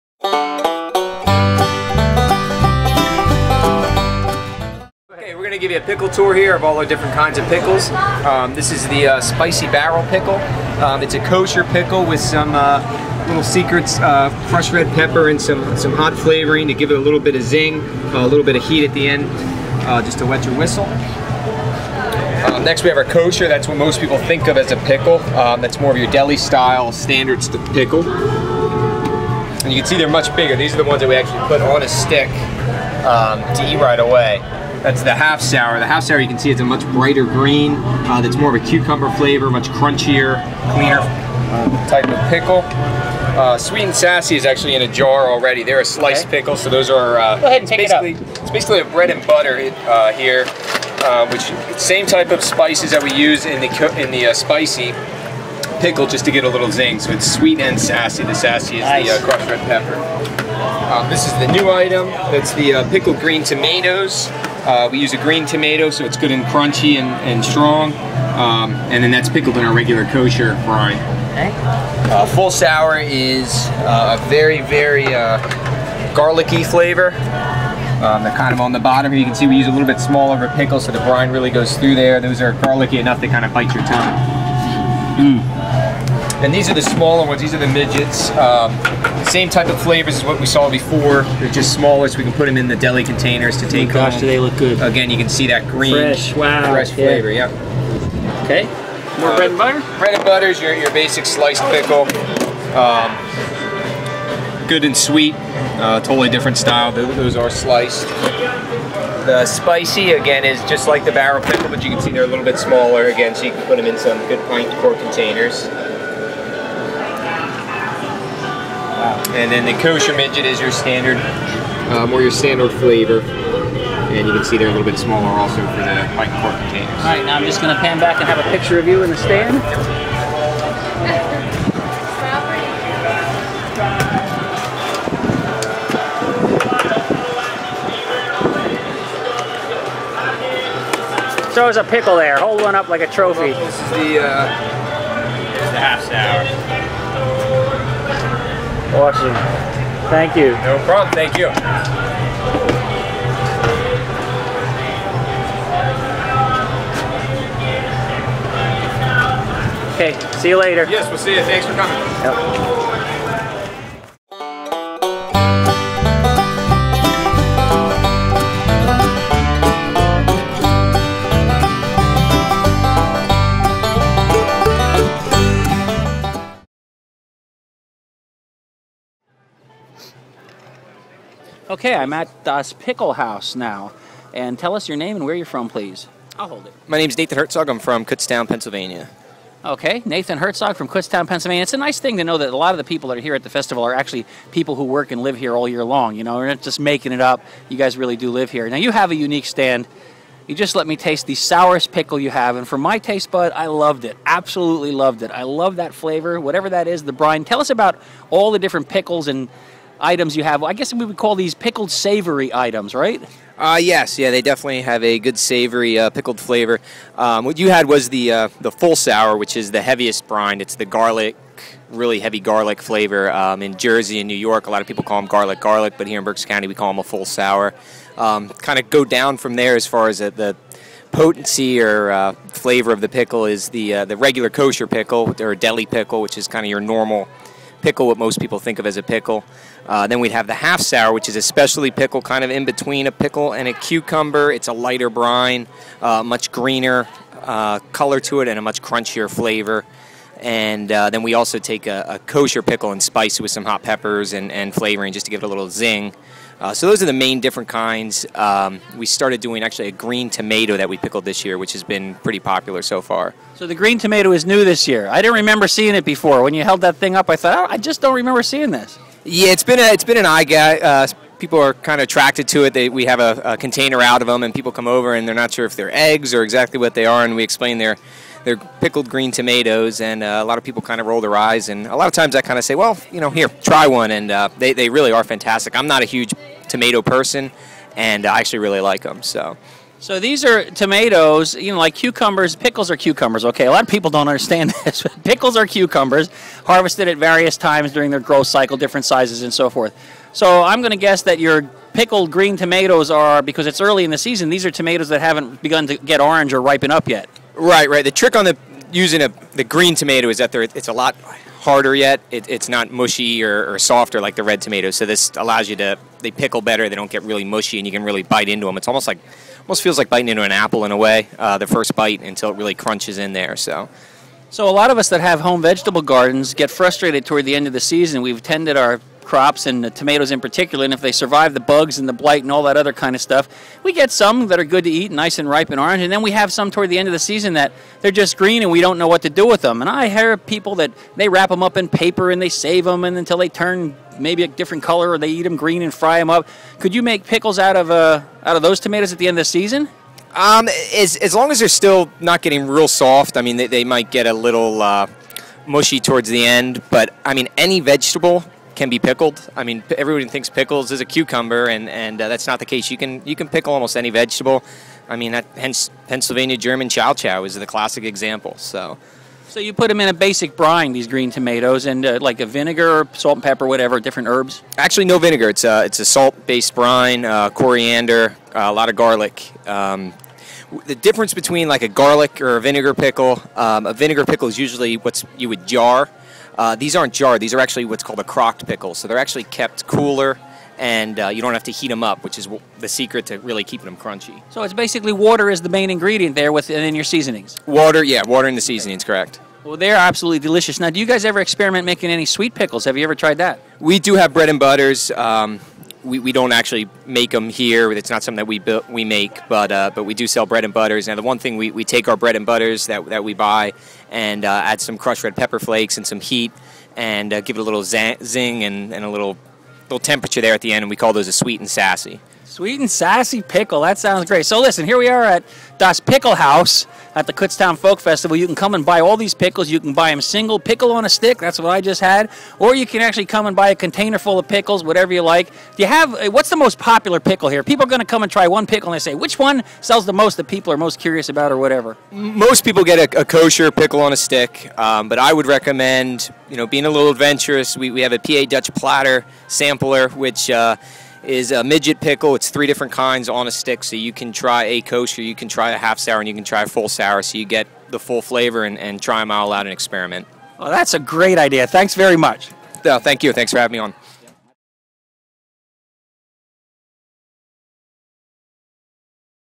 Das Pickle Haus - Kutztown Folk Festival 2013
This video features Nathan Hertzog from Das Pickle Haus at the Kutztown Folk Festival showing us all of the different styles of delicious home-made pickles and savory items he offers at the festival.  When the video segment ends, a radio interview with Nathan conducted by "East Side" Dave Kline from the Kutztown Folk Festival, WEEU Radio, Berks Country Magazine (Reading Eagle Company), and the Mountain Folk Radio & TV show follows.